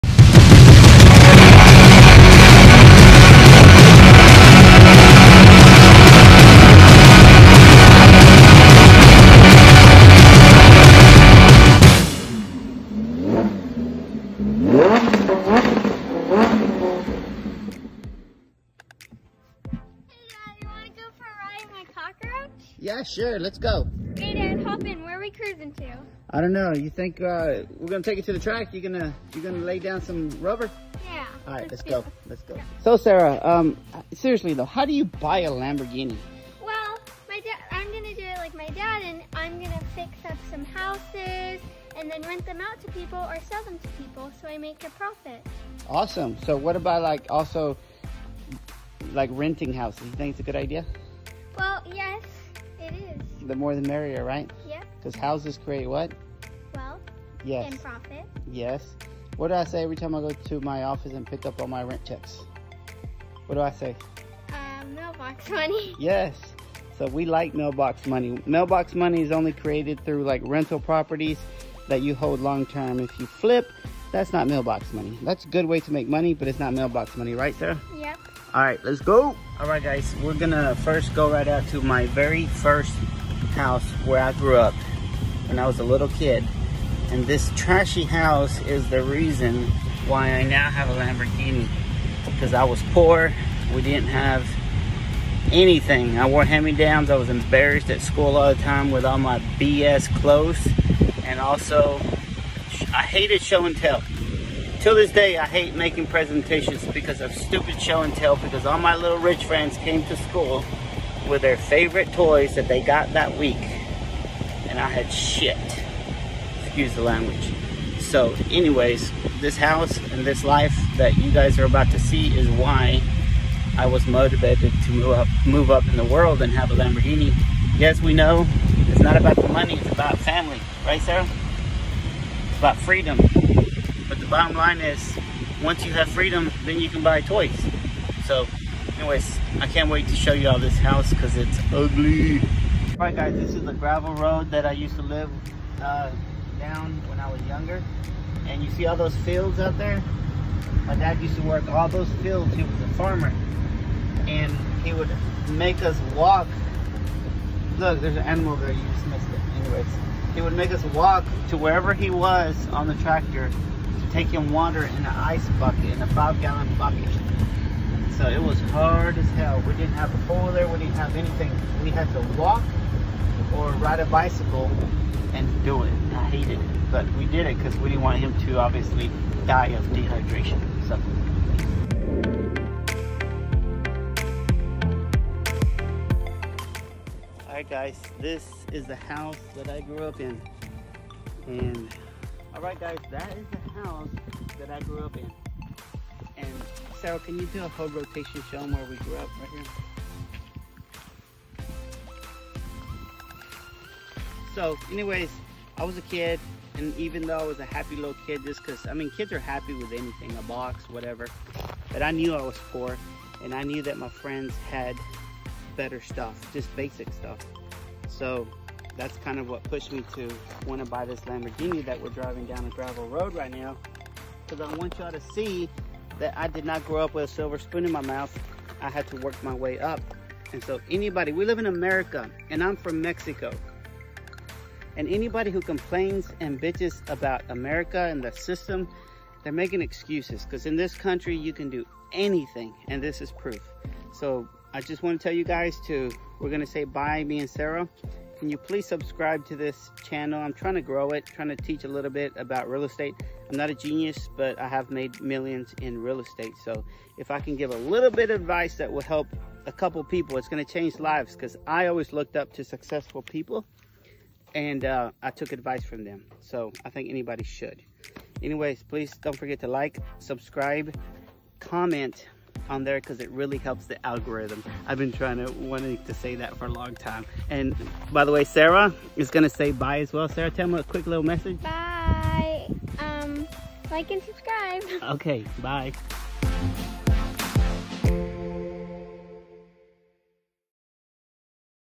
Spoiled 10 year old in Lambo!
My daughter know a lot about real estate. I teach her everyday!  So if you think she’s spoiled then you are wrong. Let her teach you something about how to make money in real estate. I came up from poverty so I teach my kids the value of smart and hard work.